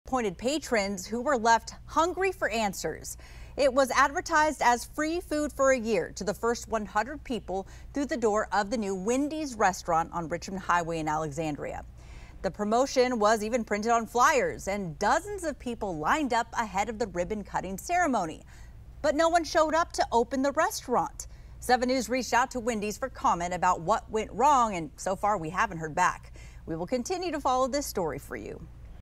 Wendy's 'no shows' it's own grand opening
Dozens of excited people lined up outside the restaurant's doors located at 6634 Richmond Highway in the Beacon Center shopping center for the Wendy's location's opening. However, no one from the restaurant showed up to open the doors, leaving everyone quite disappointed.